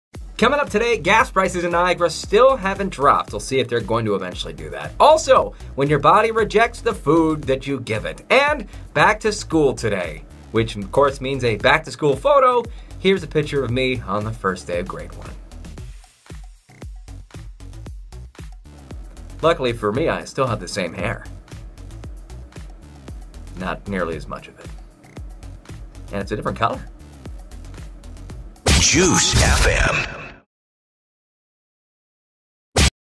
The Barn Show - Sept 5/17
Gas prices SOAR in Niagara!  Food doesn't stay where it should!

Back to School?  Let's take it WAY back!